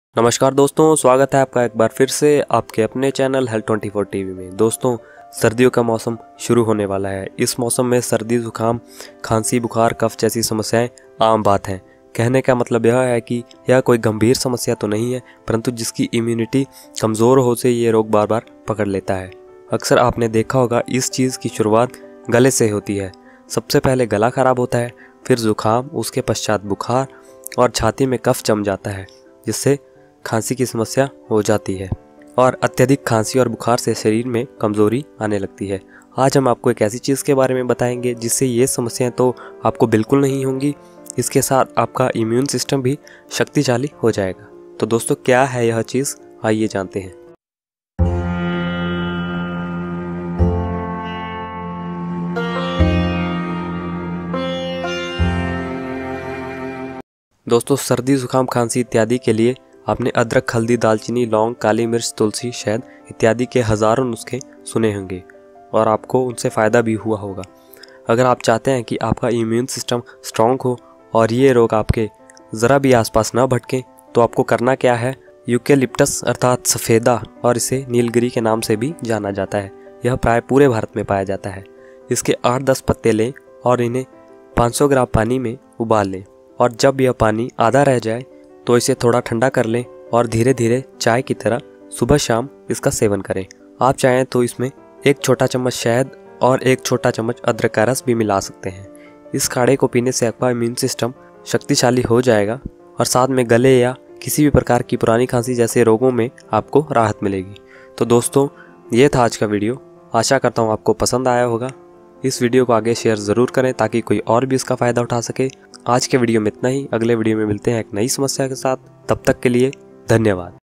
सर्दी, जुखाम, खांसी, बुखार के लिए रामबाण औषधि || Best Home Remedy For Cold And Flu || Immune System
Best home remedies for cold and flu and make immune system strong.

In the time of winter cold and flu is very common problem. In this video I am going to share with you some effective home remedies to get rid of cough or Natural Cough Remedies for Persistent. Mainly it will  Caused by viral infection, the common cold is characterized by runny nose, sneezing, coughing, sore throat, and inflammation of the mucous membrane in the nose and throat.

Remedy- Boil 8-10 leaves of eucalyptus with 500 ml water when it left half of quantity take down it from flame and take this boiled water as tea it will helps you to get rid out from cold and flu.
I hope this information will works well for you and you will take a complete benefit of this information.